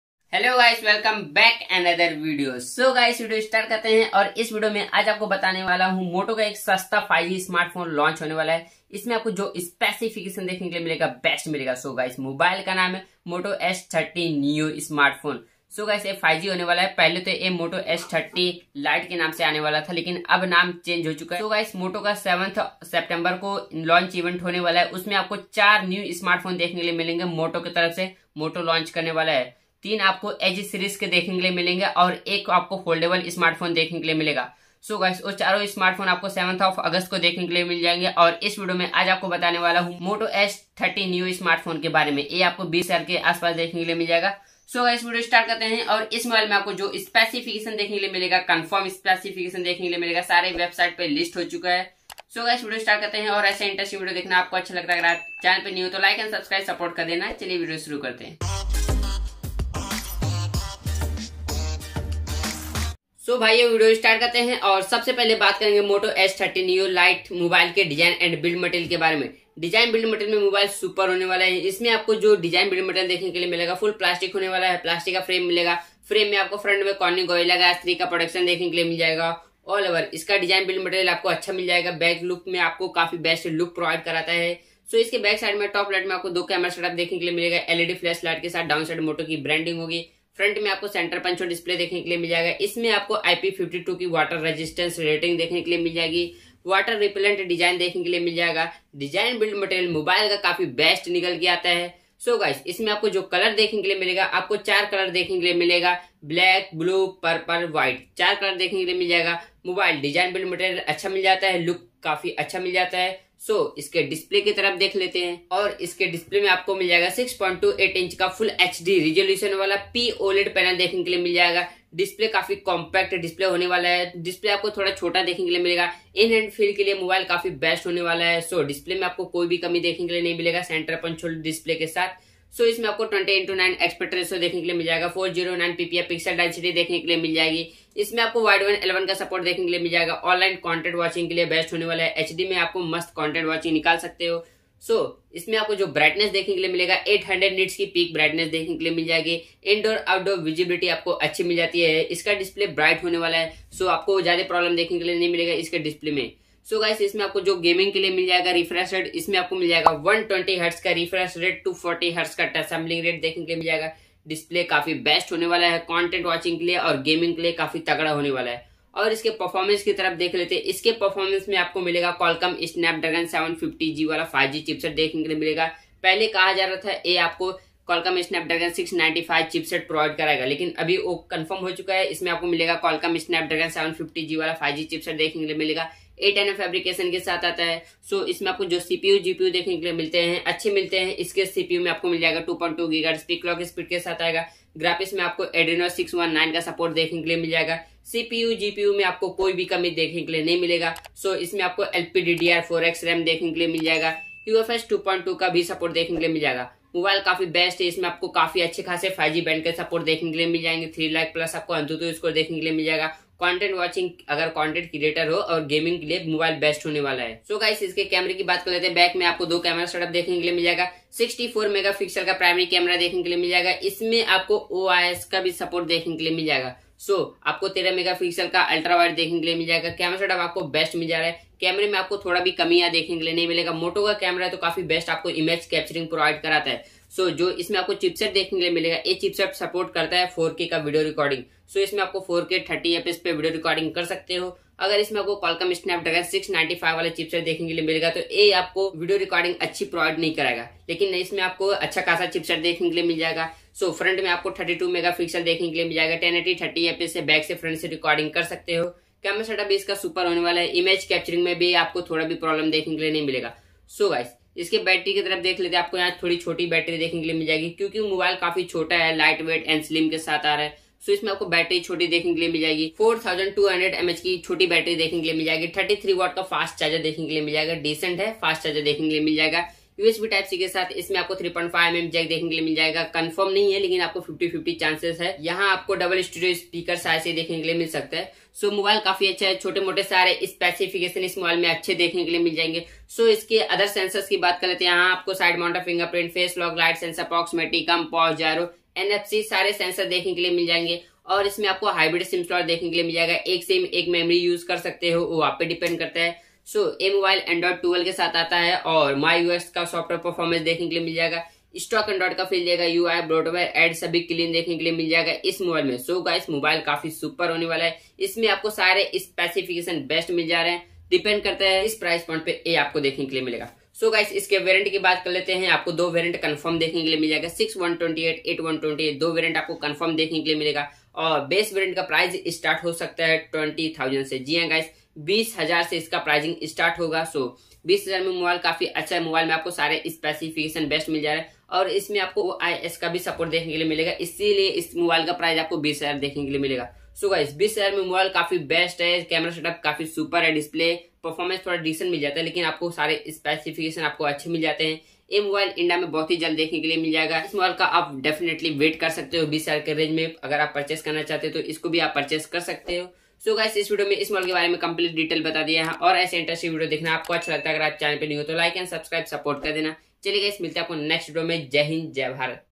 Moto Edge 30 Neo - First Look | 64MP OIS Camera | Moto Edge 30 Neo 5G Specification, Price, Review
Hindi
pro tech technology
Upcoming Phone

About : Pro Tech Technology, is a YouTube Channel, where you will find technological videos in Hindi